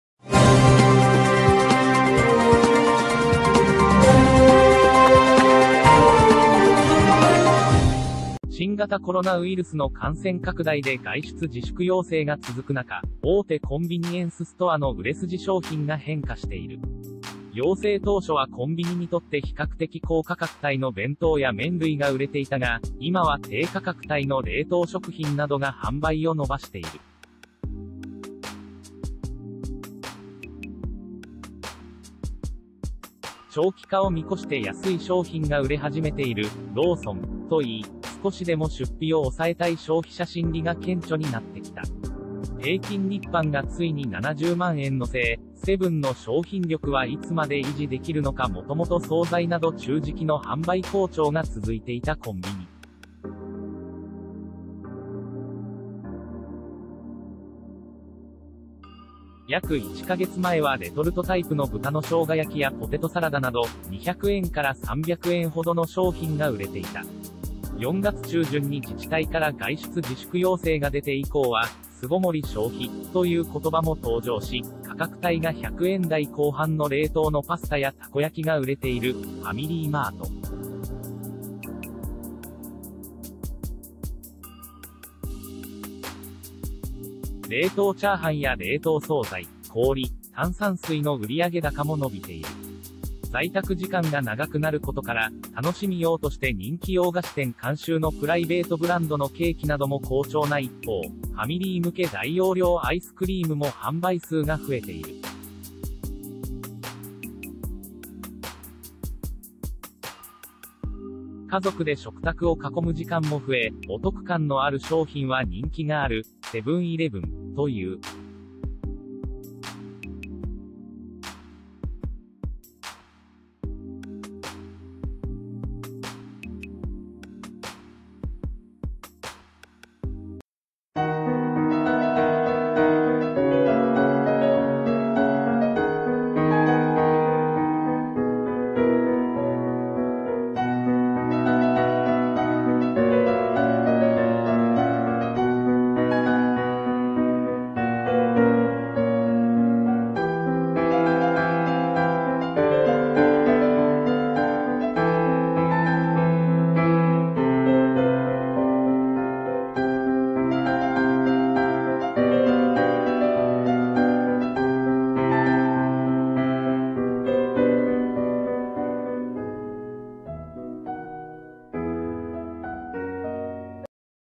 【新型コロナ】コンビニ消費に変化、安い冷凍食品と大容量アイスが売れ始めたワケ
新型コロナウイルスの感染拡大で外出自粛要請が続く中、大手コンビニエンスストアの売れ筋商品が変化している。要請当初はコンビニにとって比較的高価格帯の弁当や麺類が売れていたが、今は低価格帯の冷凍食品などが販売を伸ばしている。「長期化を見越して安い商品が売れ始めている」（ローソン）といい、少しでも出費を抑えたい消費者心理が顕著になってきた。平均日販がついに70万円乗せへ、セブンの商品力はいつまで維持できるのか　もともと総菜など中食の販売好調が続いていたコンビニ。約1カ月前はレトルトタイプの豚の生姜焼きやポテトサラダなど、200円から300円ほどの商品が売れていた。4月中旬に自治体から外出自粛要請が出て以降は「巣ごもり消費」という言葉も登場し、「価格帯が100円台後半の冷凍のパスタやたこ焼きが売れている」（ファミリーマート）。冷凍チャーハンや冷凍総菜、氷、炭酸水の売上高も伸びている。  　在宅時間が長くなることから、楽しみ用として人気洋菓子店監修のプライベートブランドのケーキなども好調な一方、ファミリー向け大容量アイスクリームも販売数が増えている。家族で食卓を囲む時間も増え、「お得感のある商品は人気がある」（セブン―イレブン）という。